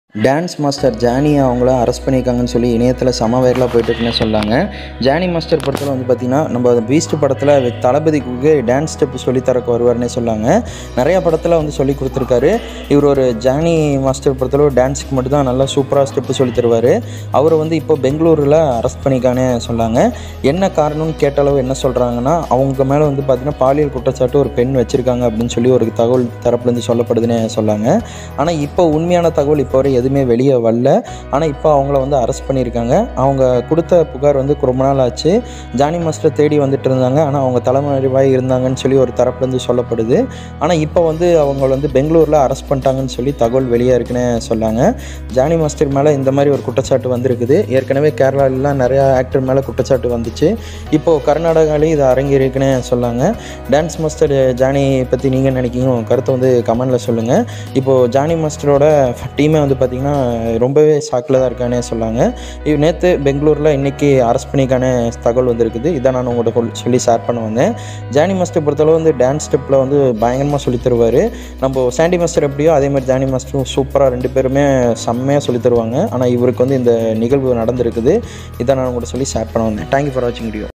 Hyderabad டு Bangalore... களமிறங்கிய 'Special போலீஸ் டீம்'.. தலைமறைவான ஜானி மாஸ்டர் போக்சோவில் கைது!
Dance Director Johnny master Arrest Bangalore| Shooking update Beast Movie Dance Step Thalapathy